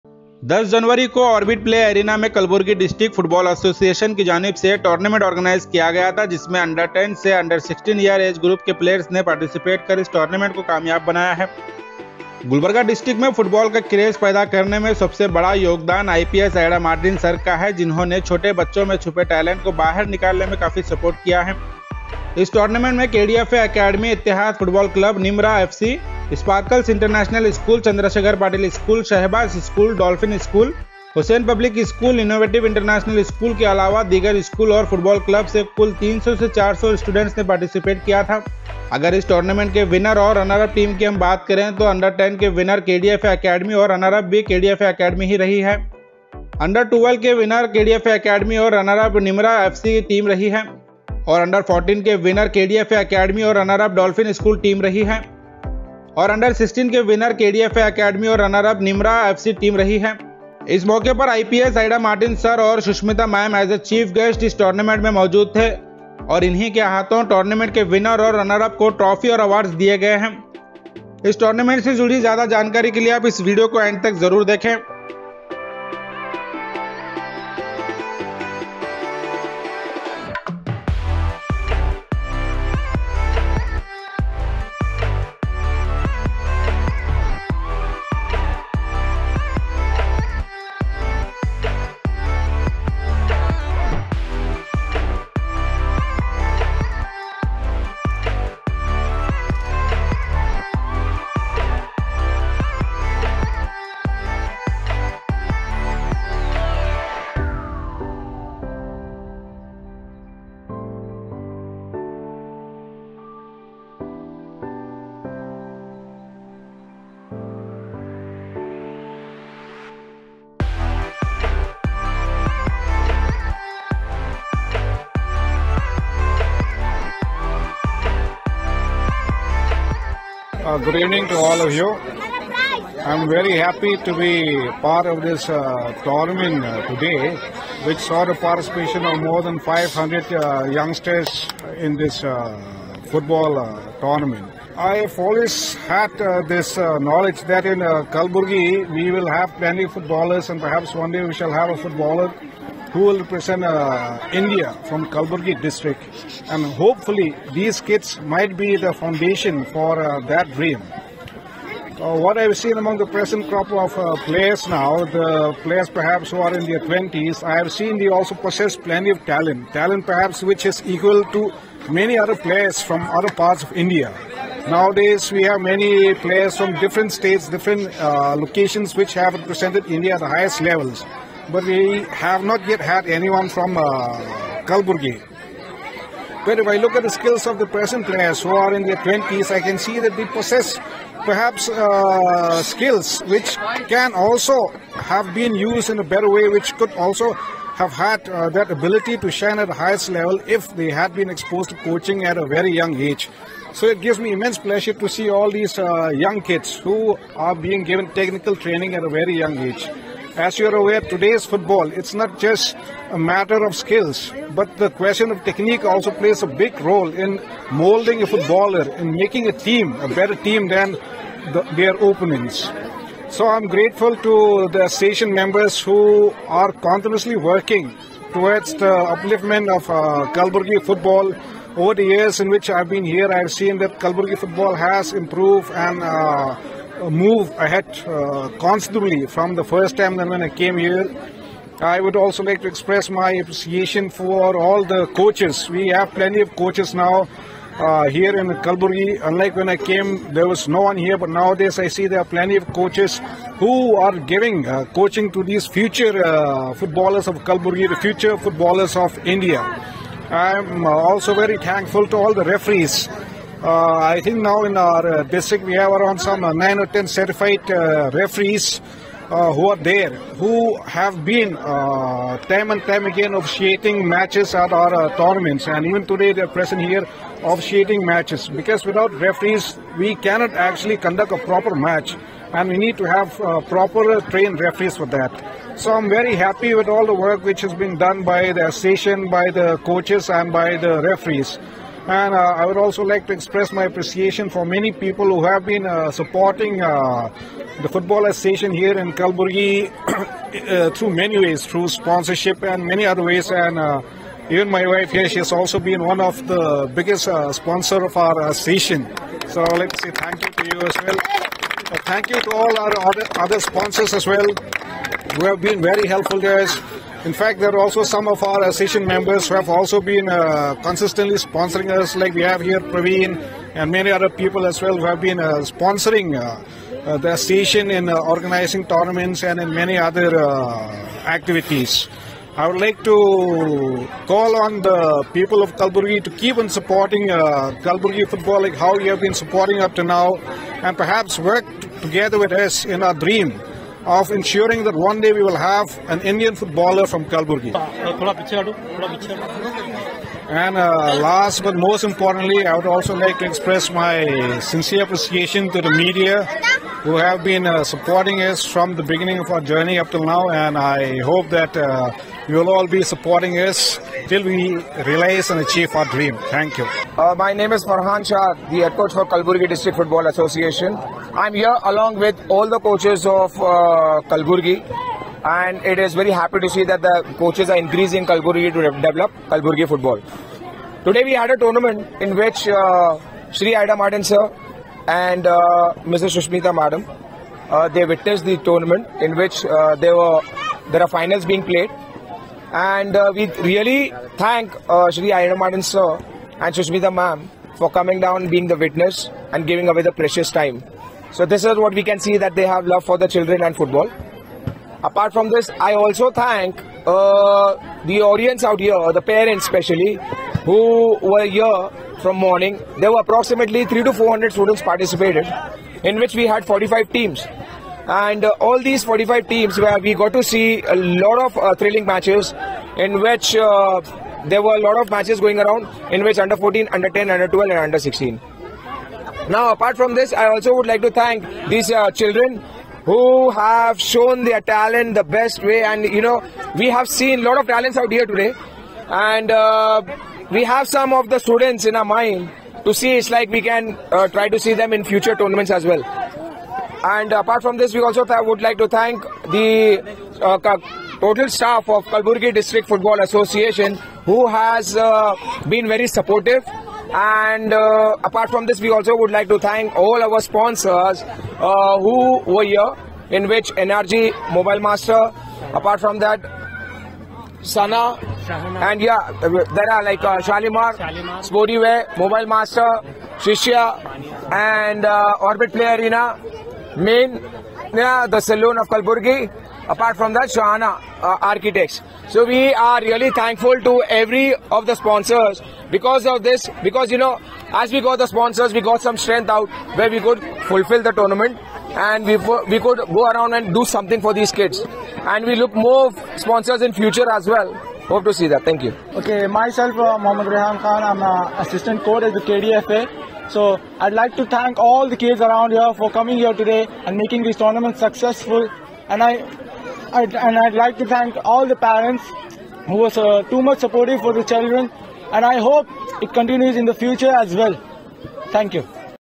45 Footballs Team And More Then 300 Players Are Participated In Football Tournament | KDFA
45 Football Team And More Then 300 U10 To U16 Age Group Players Are  Participated In  Football Tournament Organised By KDFA Academy On 10 January